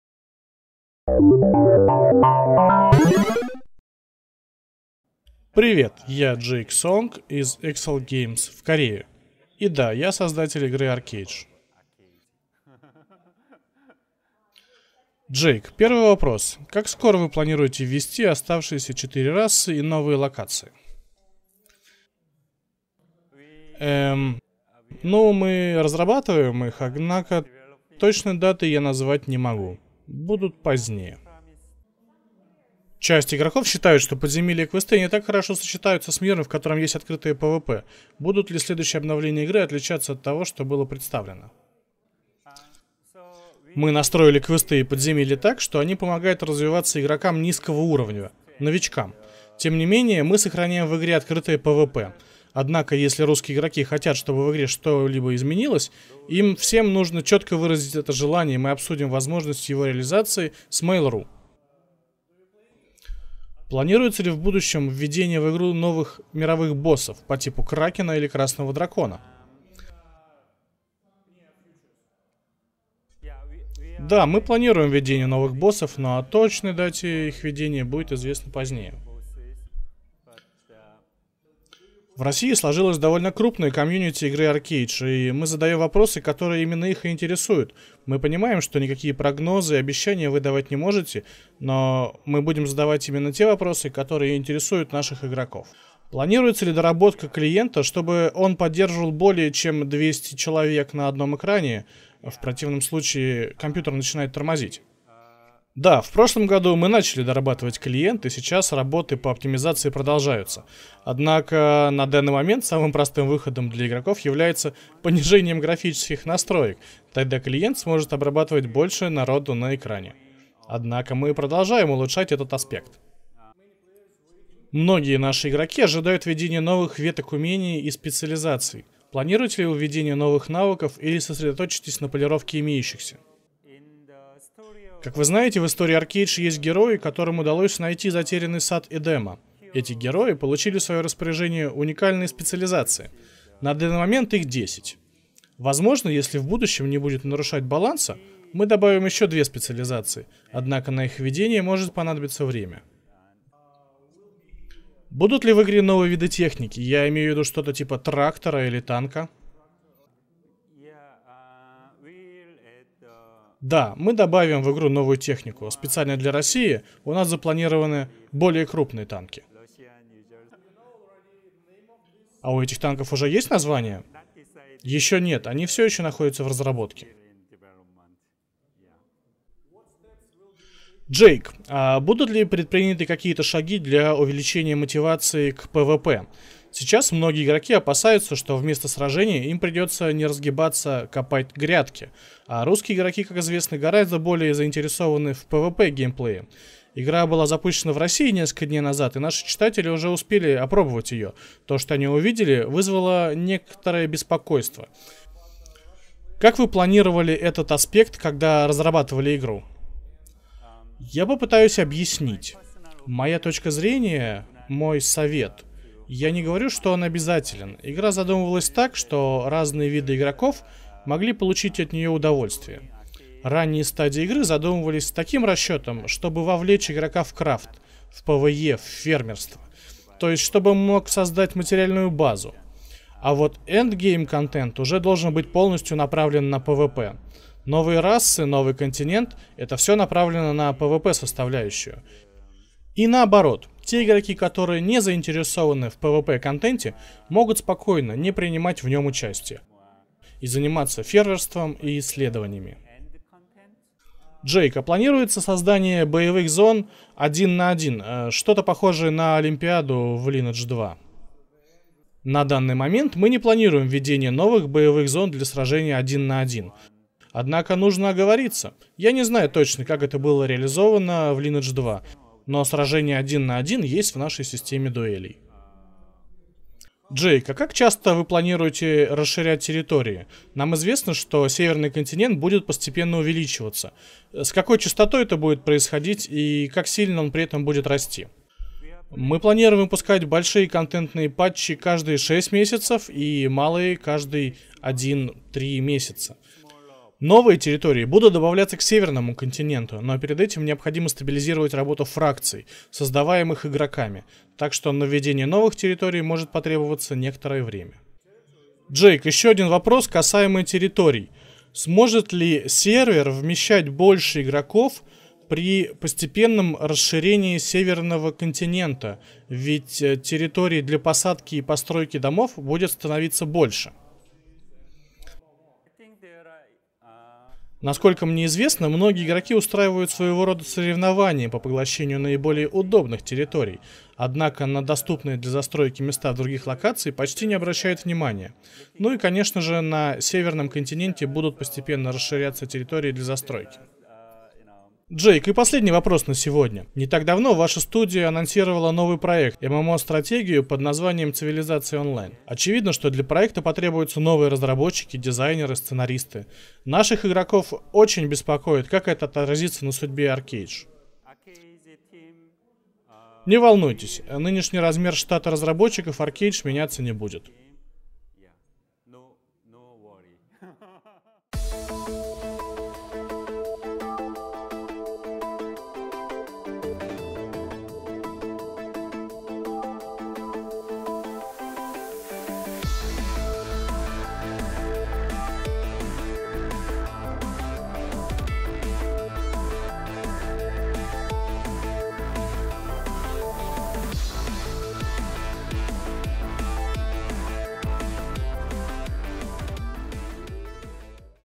GoHa.Ru | ArcheAge - Интервью с Джейком Сонгом
Интервью создателя MMORPG ArcheAge Джейка Сонга порталу GoHa.Ru. Вам предстоит узнать информацию о: расах и локациях, подземельях, мировых боссах, умениях, технике, дуэлях, территории и т.д.